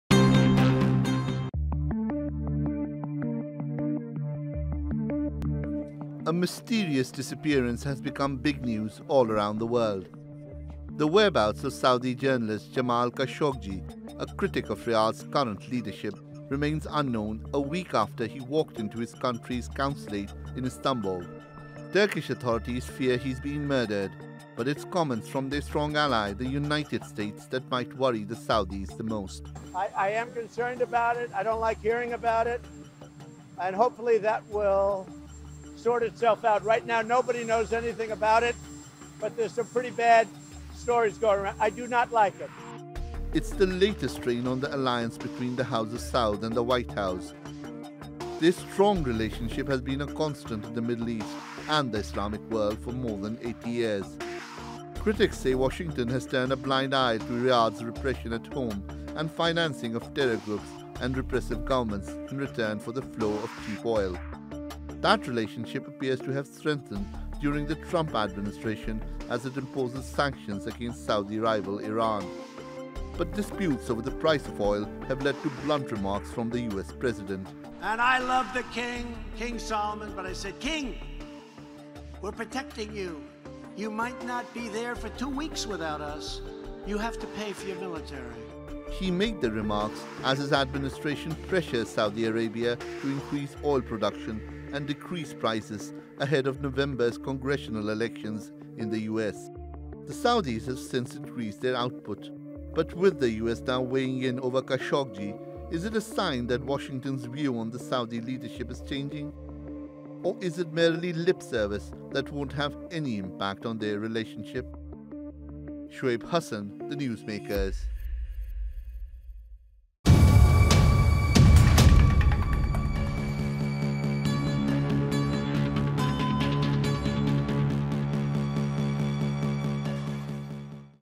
Could Saudi Arabia survive without US?
US President Donald Trump has expressed concerns over Saudi journalist Jamal Khashoggi's whereabouts. Riyadh insists he safely left the consulate here in Istanbul last week. But American officials are urging a transparent investigation into what happened to him. The incident is the latest strain between the US and Saudi Arabia, after Trump's claims earlier this month that King Salman wouldn't remain in power for two weeks without US support. Is that accurate? Shoaib Hasan reports.